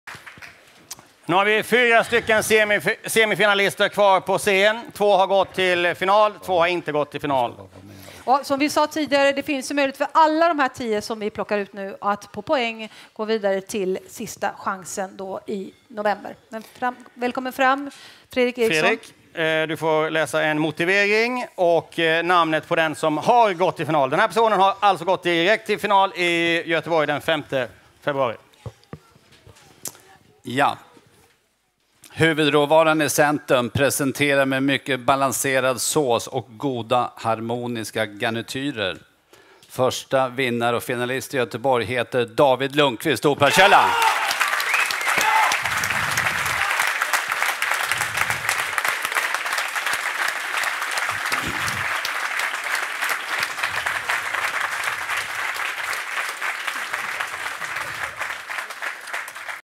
Finalist 1 David Lundqvist KLIPP
Färdig till finalen för Årets Kock 2015 i Göteborg är David Lundqvist